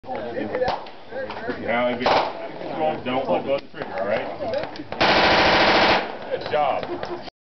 MP5 and a 9 year old
James going full auto with an MP5.